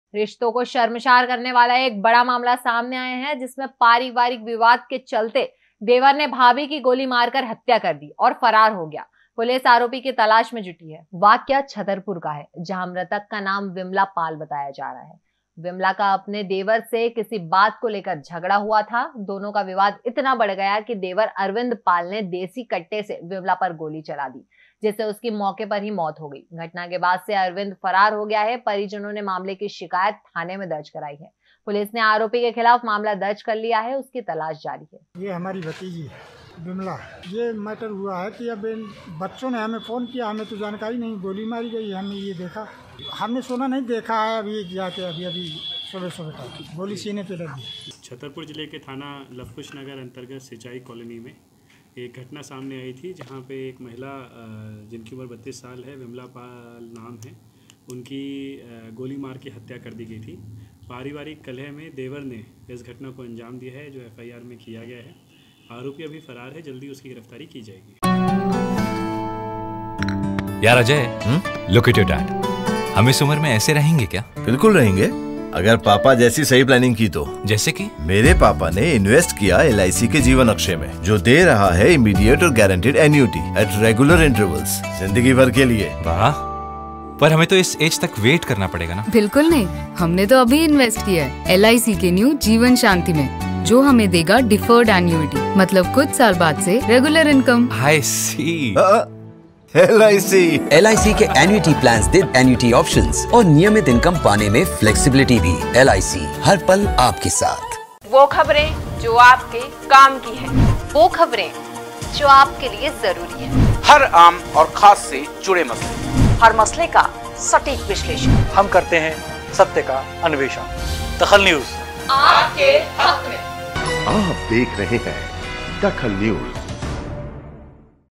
DAKHAL NEWS देवर ने की भाभी की गोली मारकर हत्या
देवर ने की भाभी की गोली मारकर हत्या
गोली मारकर फरार हुआ आरोपी देवर